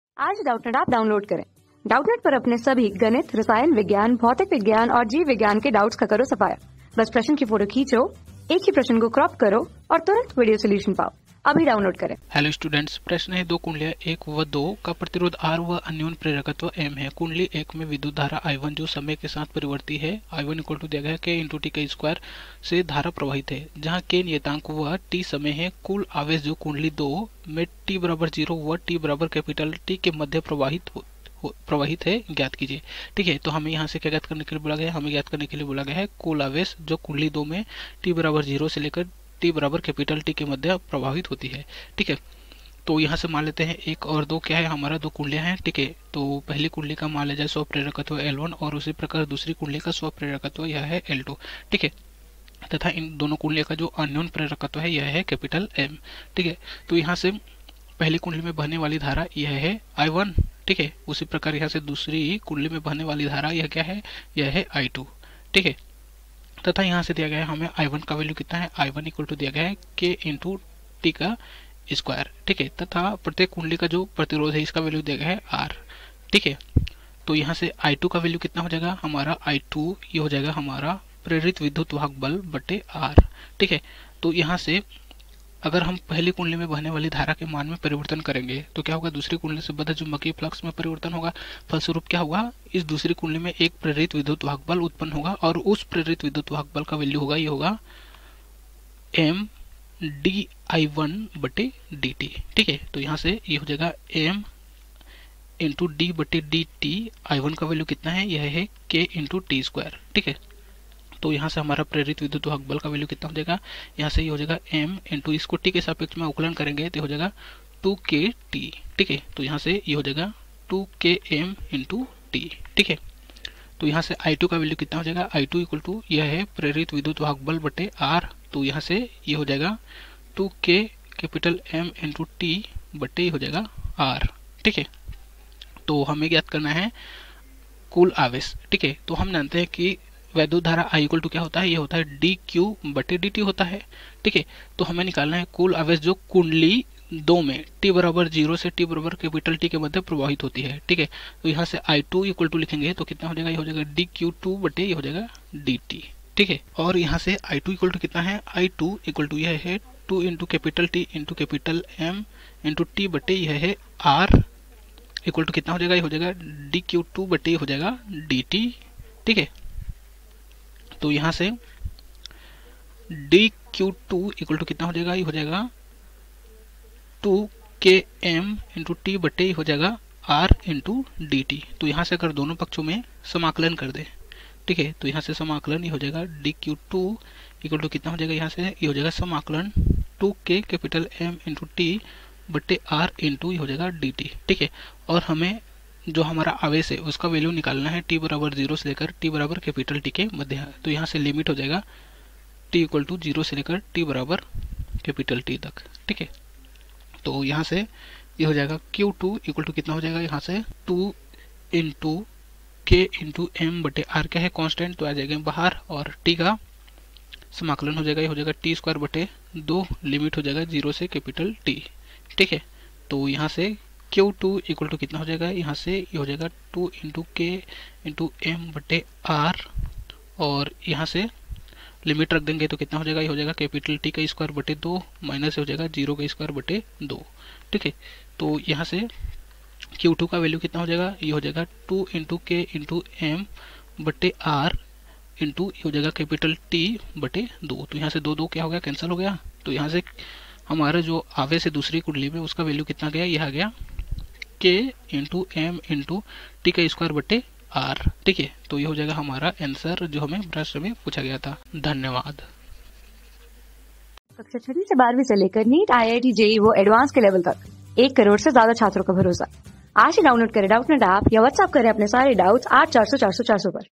दो कुण्डलियों 1 व 2 का प्रतिरोध R व अन्योन्य प्रेरकत्व M है। कुण्डली 1 में विधुतधारा I_1जो समय क...
दो कुण्डलियों 1 व 2 का प्रतिरोध R व अन्योन्य प्रेरकत्व M है। कुण्डली 1 में विधुतधारा I_1जो समय के साथ परिवर्ती है : I_1 =kt^2 , से धारा प्रवाहित है। जहाँ K नियतांक है व \'t समय है। कुल आवेश जो कुण्डली 2 में t = 0वt = T के मध्य प्रवाहित है, ज्ञात कीजिये।
Class: 12
Subject: PHYSICS
Chapter: ELECTRO MAGNETIC INDUCTION
Board:IIT JEE

👉 Have Any Query? Ask Us.